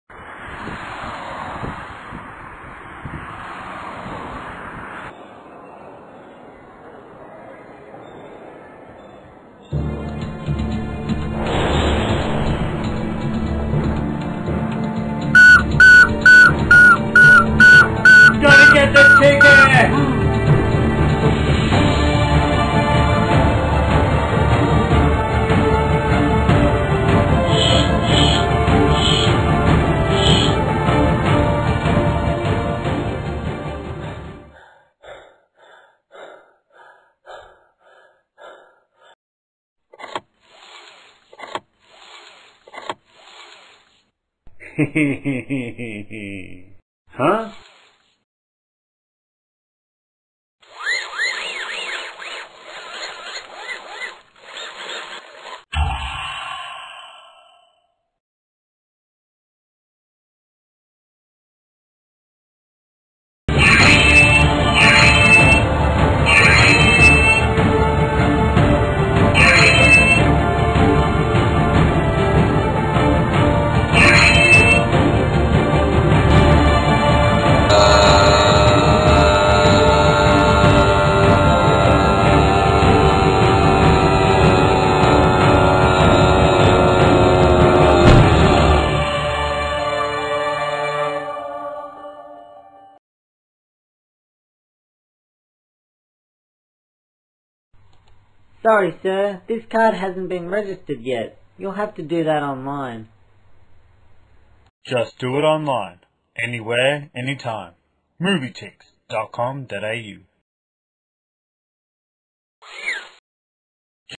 Flash animation - faux online movie ticket commercial
Assignment for multimedia authoring at Swinburne University
Created by Aaron Shiel